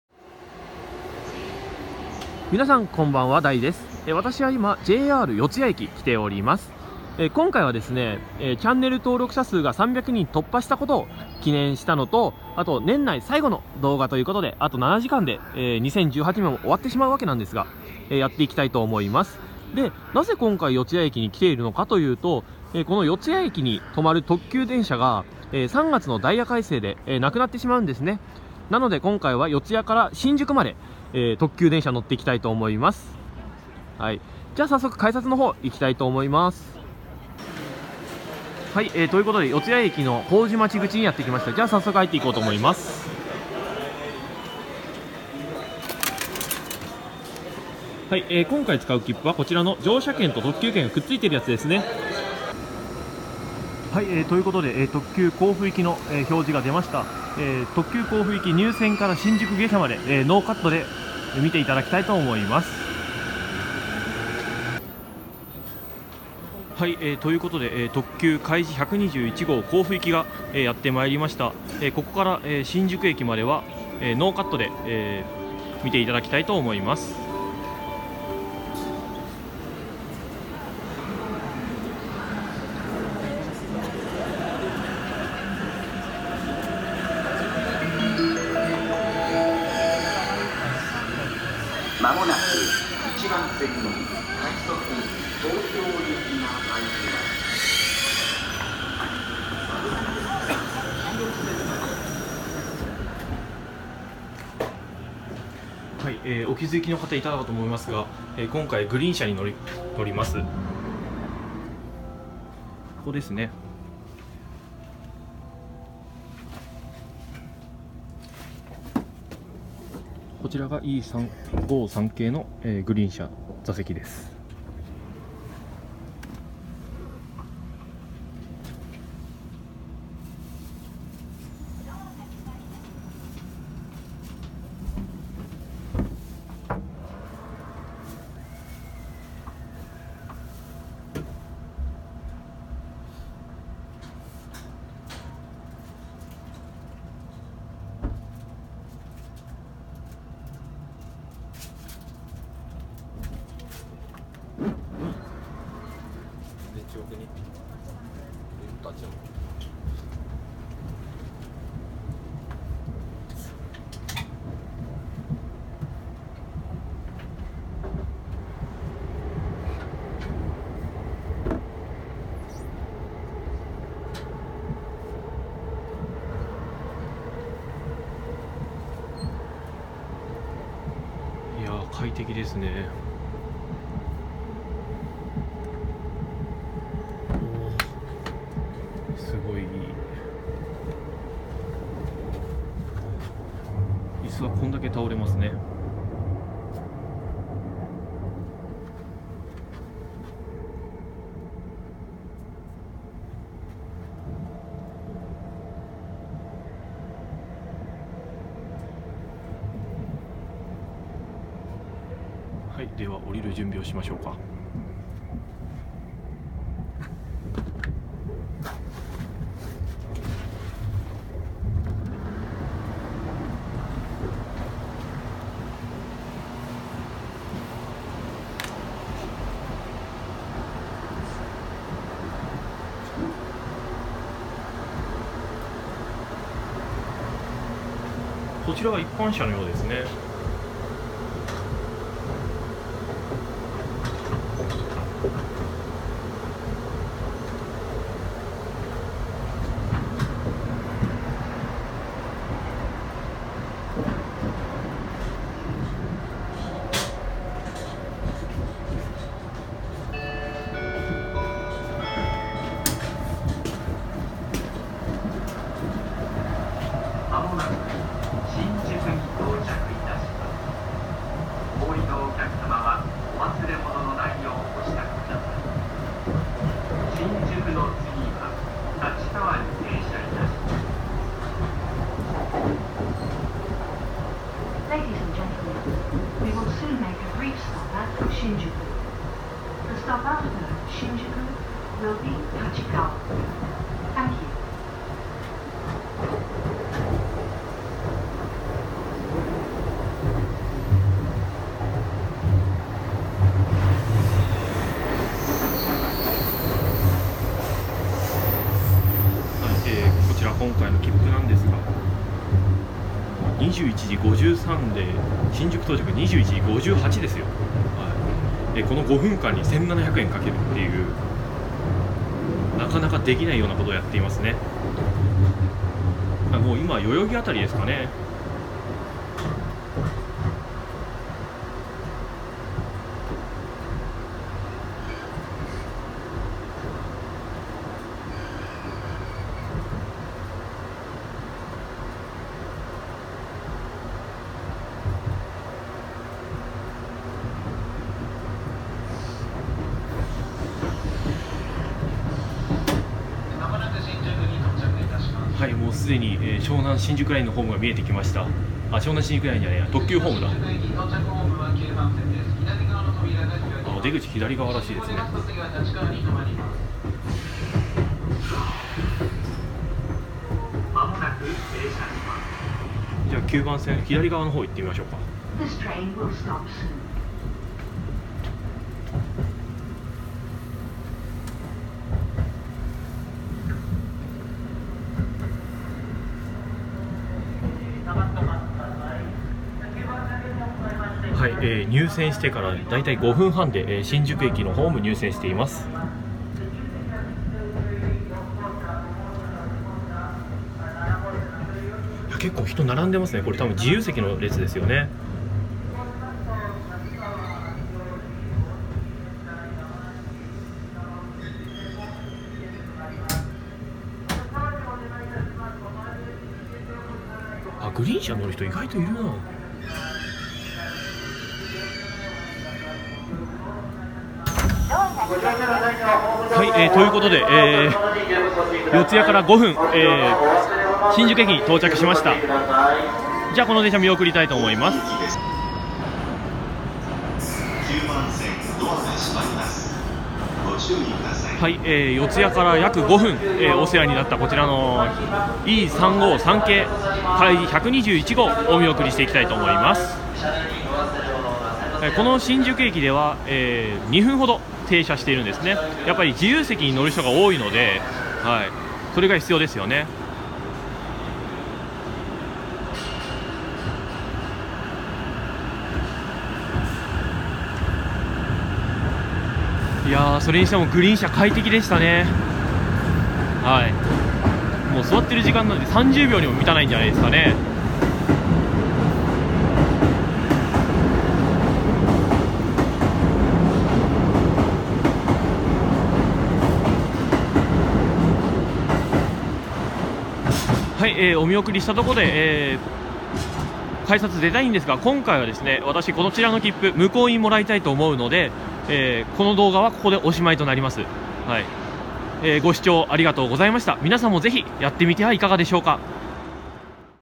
【2度と出来ないグリーン課金！】四ツ谷→新宿で特急グリーン車に乗ってきた
今回は3月のダイヤ改正で無くなる特急列車の四ツ谷停車に乗ってきました。
見ていただければわかりますが、超絶アホな企画です。
なぜかというと、四ツ谷→新宿でかいじ号のグリーン車に乗ったからですね。どこの貴族だよ。
皆さんも特急の四ツ谷停車無くなる前にご覧いただいたらいかがでしょうか？
今度は三鷹停車も行こうかな。